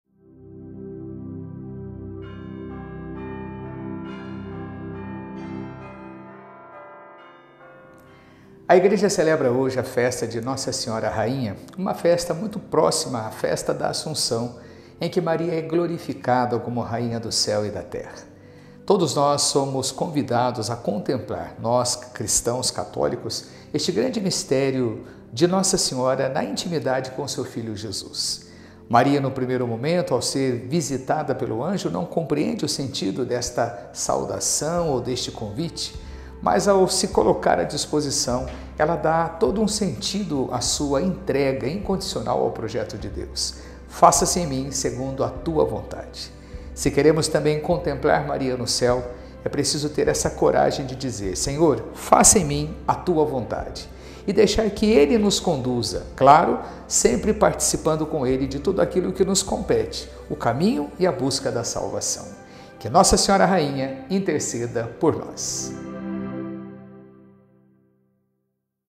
Mensagem do pároco Pe. Silvio Cesar - 22/08/23
Nossa Senhora Rainha - Memória | Terça-feira

Naquele tempo, o anjo Gabriel foi enviado por Deus a uma cidade da Galileia, chamada Nazaré, a uma virgem, prometida em casamento a um homem chamado José. Ele era descendente de Davi e o nome da Virgem era Maria.
O anjo entrou onde ela estava e disse: “Alegra-te, cheia de graça, o Senhor está contigo!” Maria ficou perturbada com estas palavras e começou a pensar qual seria o significado da saudação.
O anjo, então, disse-lhe: “Não tenhas medo, Maria, porque encontraste graça diante de Deus. Eis que conceberás e darás à luz um filho, a quem porás o nome de Jesus. Ele será grande, será chamado Filho do Altíssimo, e o Senhor Deus lhe dará o trono de seu pai Davi. Ele reinará para sempre sobre os descendentes de Jacó, e o seu reino não terá fim”.
Maria perguntou ao anjo: “Como acontecerá isso, se eu não conheço homem algum?” O anjo respondeu: “O Espírito virá sobre ti, e o poder do Altíssimo te cobrirá com sua sombra. Por isso, o menino que vai nascer será chamado Santo, Filho de Deus. Também Isabel, tua parenta, concebeu um filho na velhice. Este já é o sexto mês daquela que era considerada estéril, porque para Deus nada é impossível”.
Maria, então, disse: “Eis aqui a serva do Senhor; faça-se em mim segundo a tua palavra!” E o anjo retirou-se.